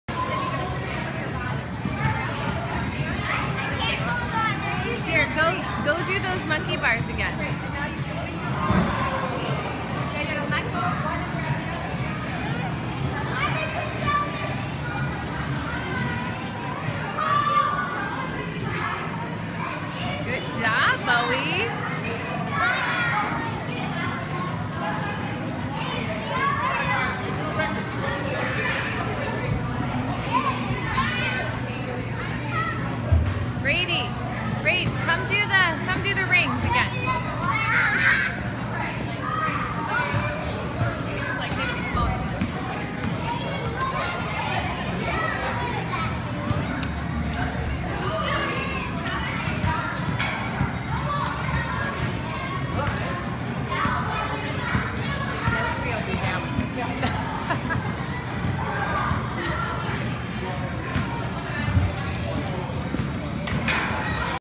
Junior Ninja Warriors
The boys had the best time at a ninja warrior birthday party this weekend! Brady was especially in his element! What a great party!